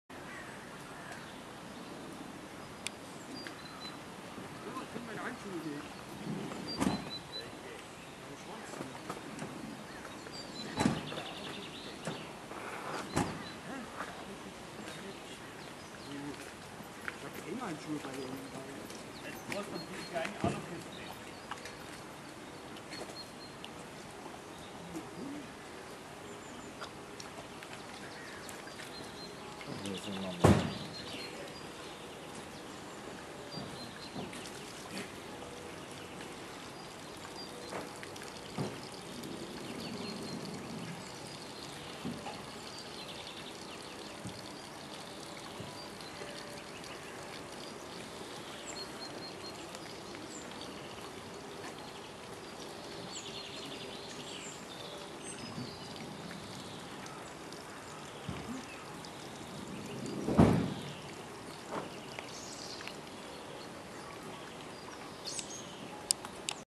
アカカワイノシシ
アカカワイノシシのおしっこは長い

2013年3月18日　ミュンヘンzooにて